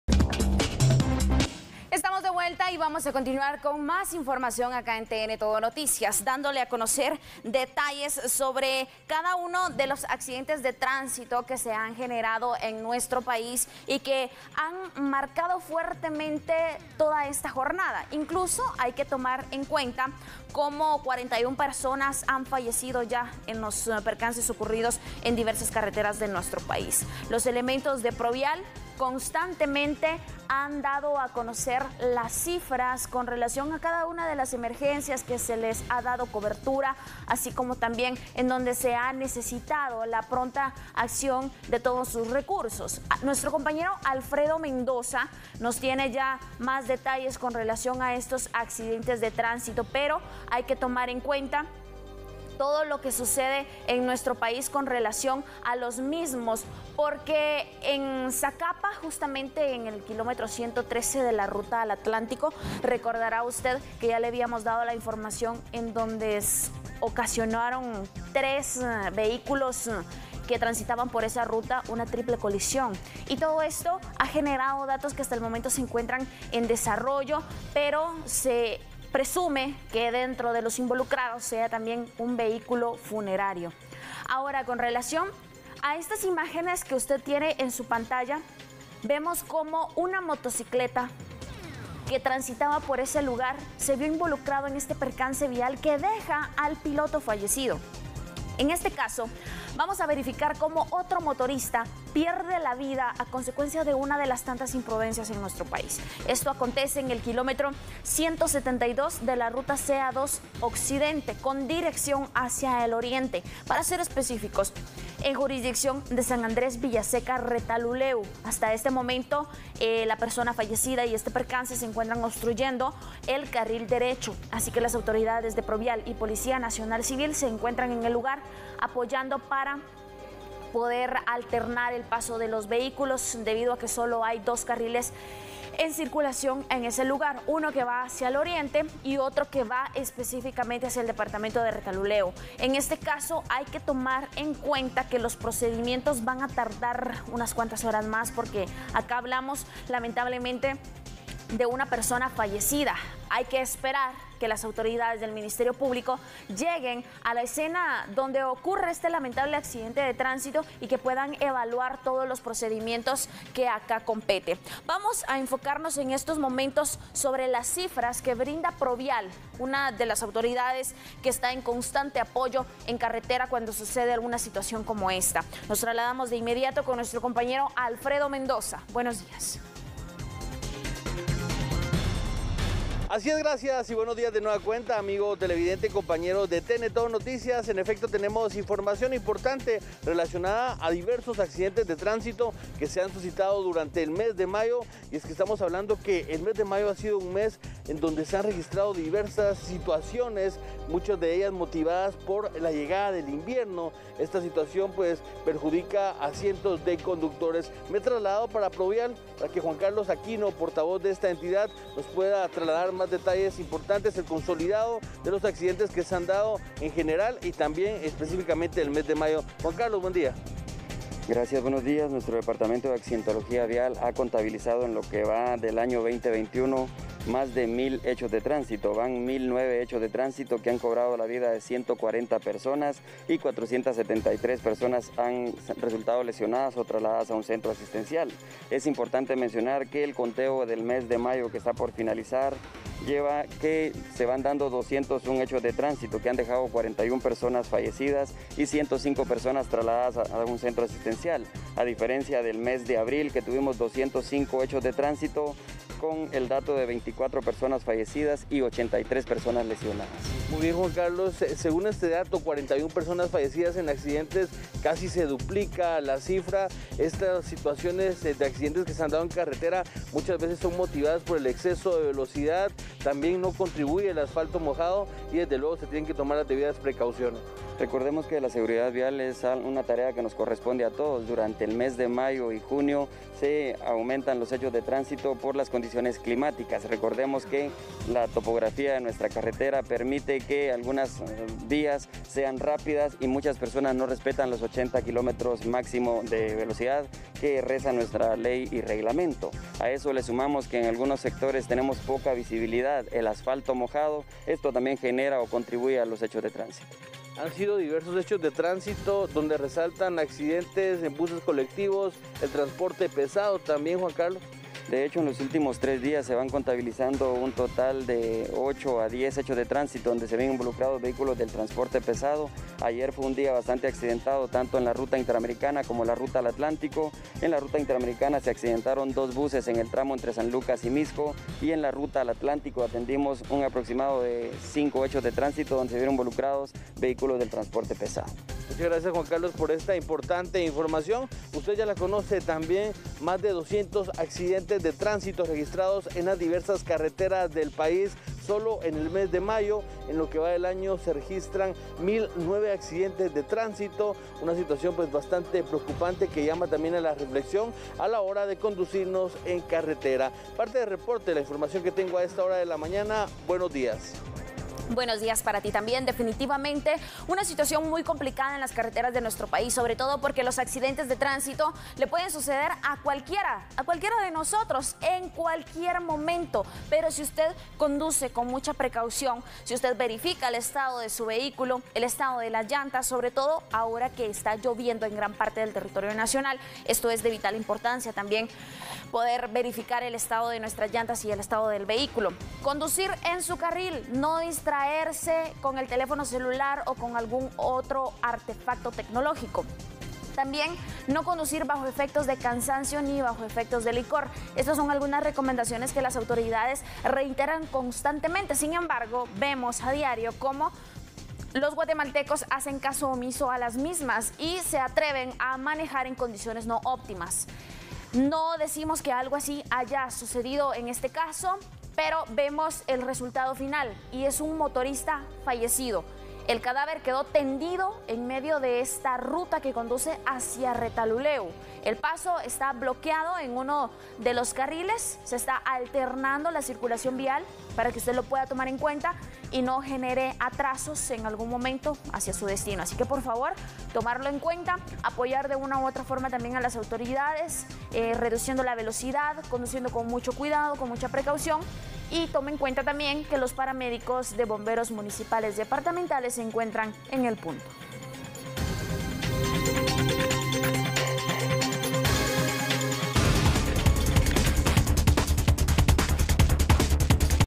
Constantes accidentes de tránsito en los últimos días
Hace instantes un motociclista falleció al sufrir percance vial